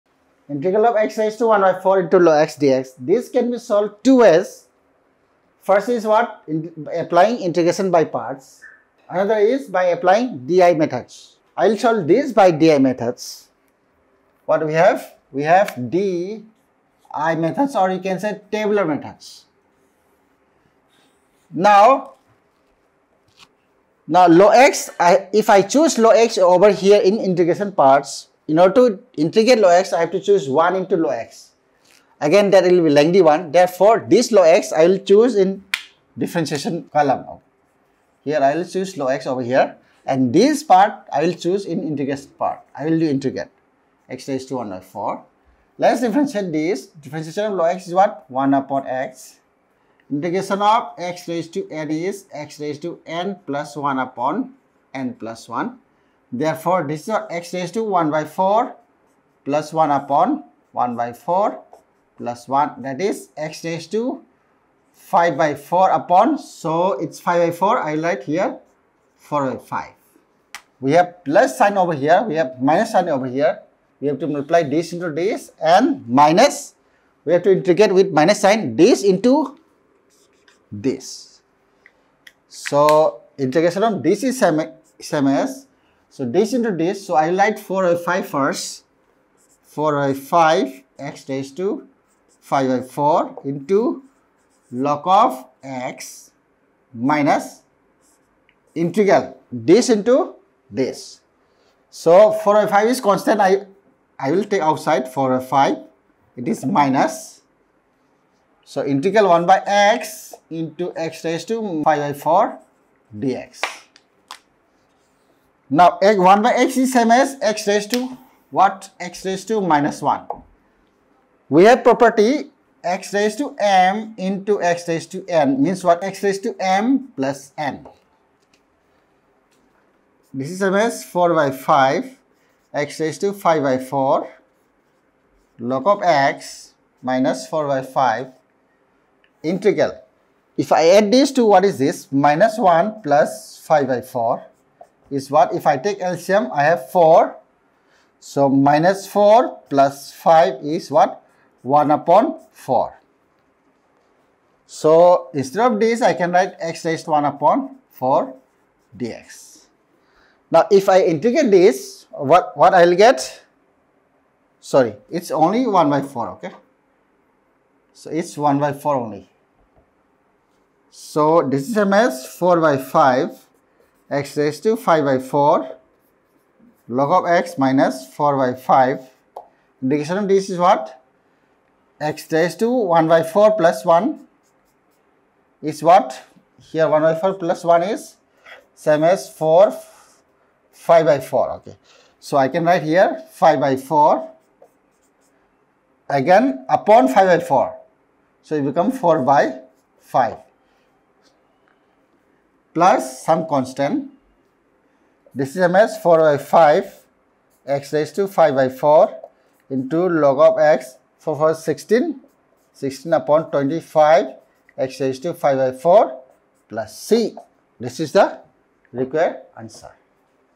MIT Integration 2012 Q2
Solving integral of x^(1/4)logx

Please Help me create more free content!